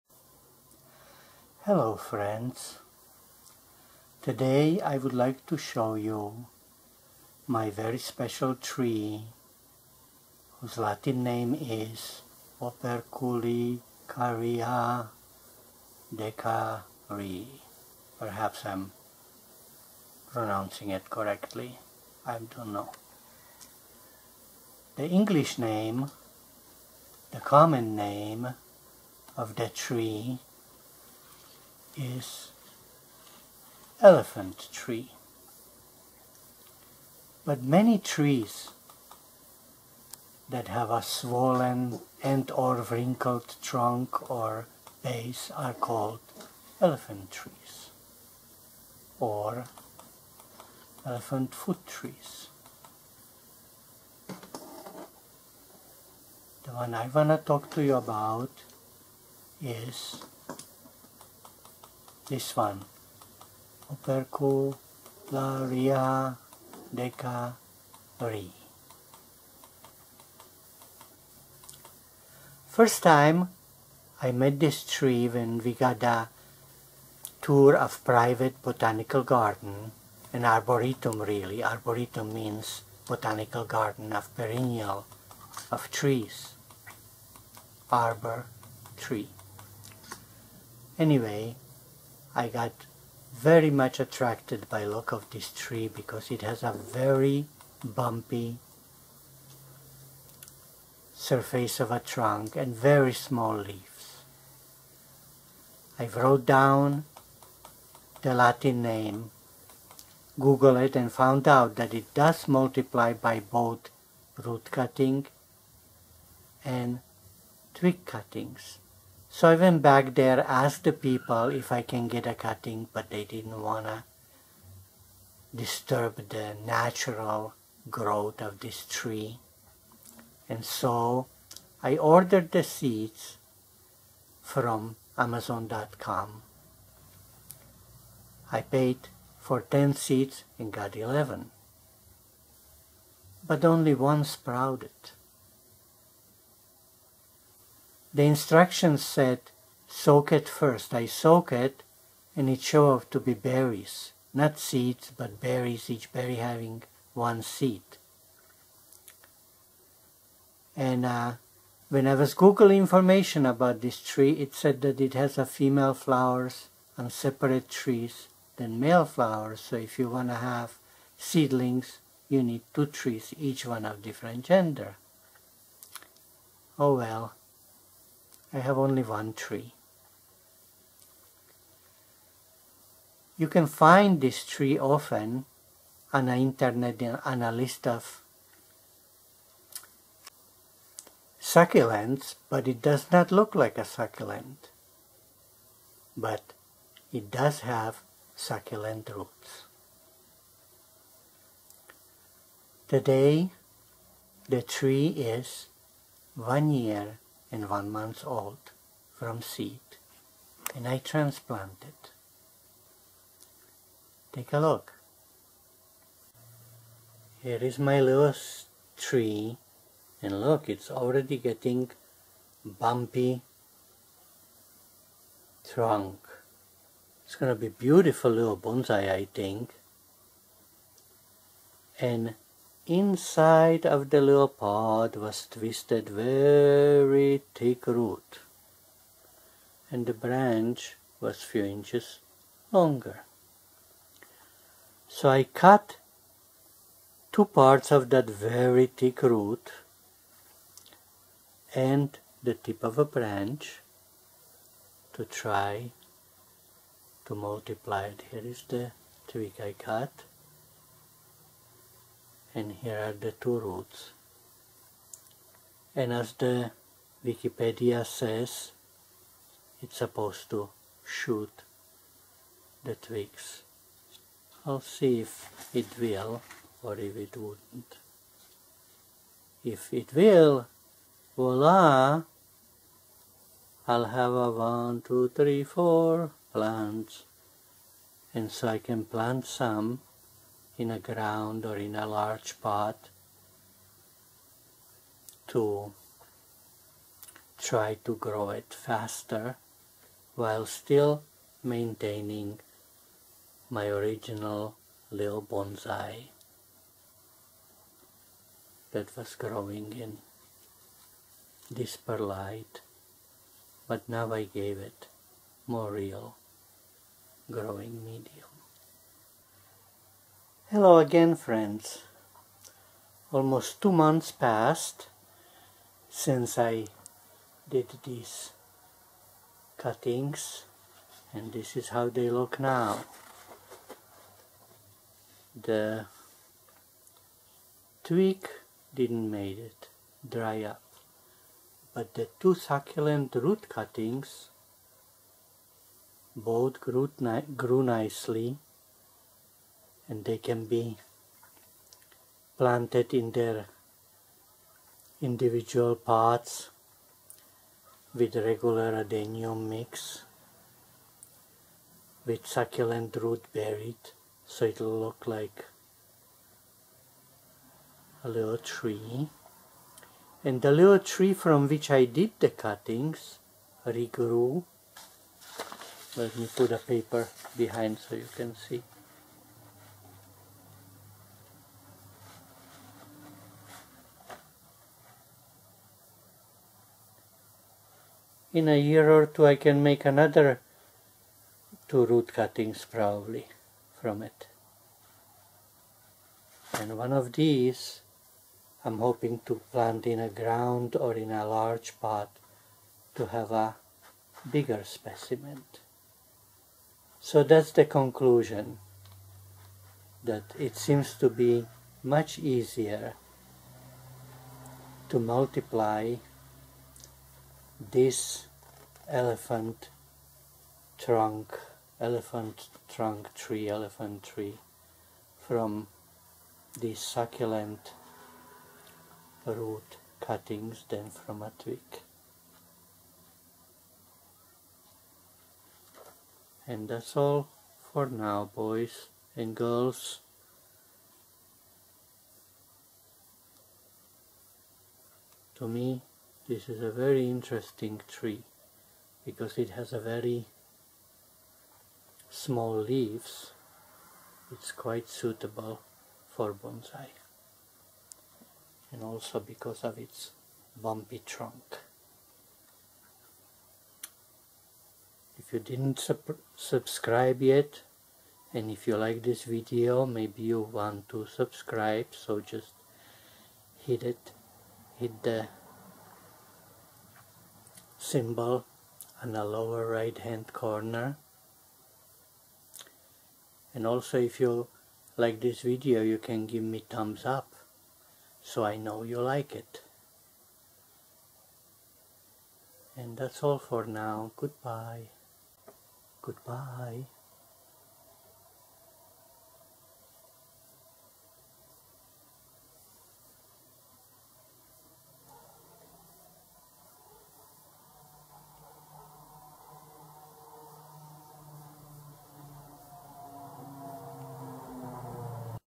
How to grow Elephant tree - Operculicarya decaryi - mini bonsai and propagation
How to grow Elephant tree - Operculicarya Decaryi - from seed or twig cutting or root cutting - my experiences.
This cute specie is very suitable for bonsai or potted plant, because has VERY small leaves and bumpy trunk, giving it that miniature aged tree look and because it stores water under ground in roots, if You forget to water it'll access that storage so nothing bad happens :-)
Cheers!